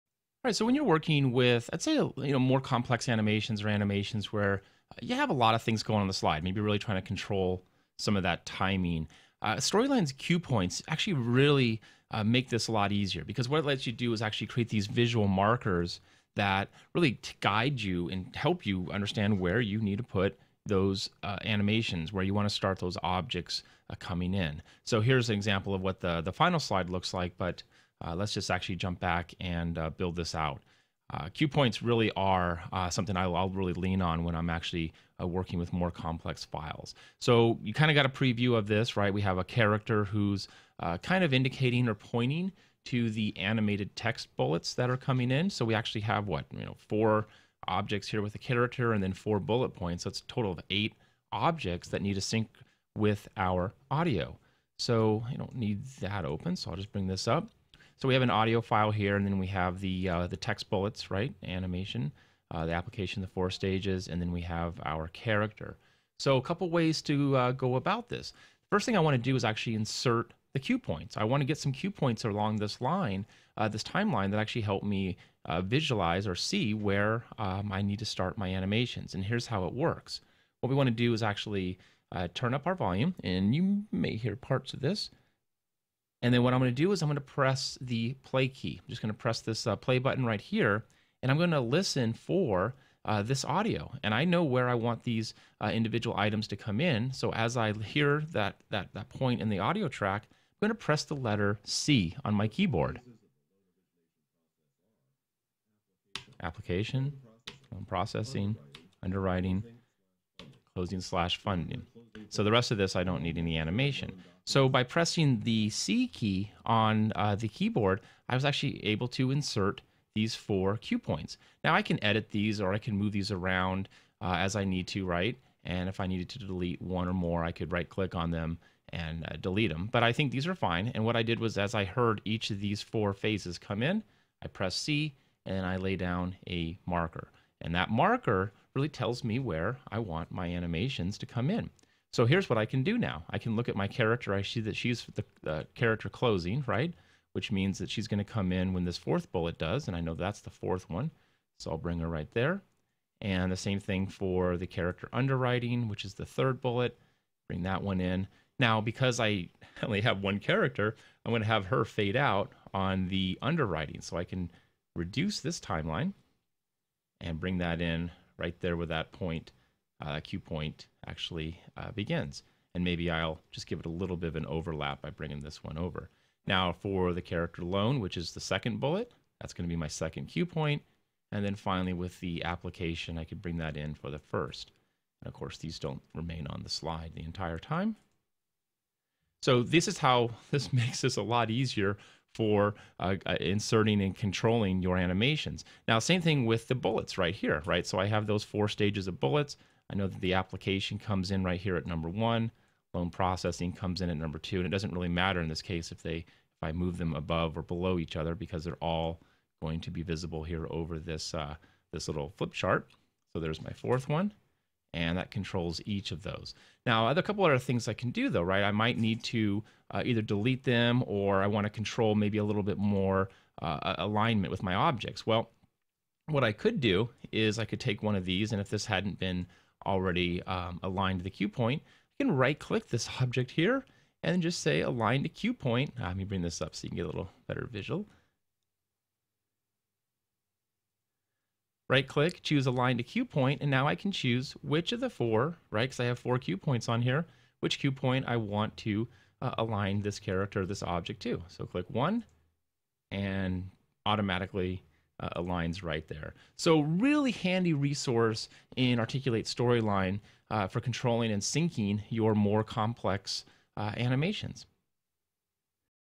Articulate Storyline tutorial: Syncing animations with cue points
In this tutorial we'll show you how to synchronize audio and animation in your e-learning course. Get more Articulate tips, guides and templates

Well-synchronized audio and animation can give learners that smooth flow you want in your course. To synchronize them perfectly, try the cue points feature in Articulate Storyline. This video shows how easy it is to use cue points to deliver a seamless multimedia experience to your learners.

Articulate makes Storyline and Studio, the world's favorite e-learning authoring tools. for more.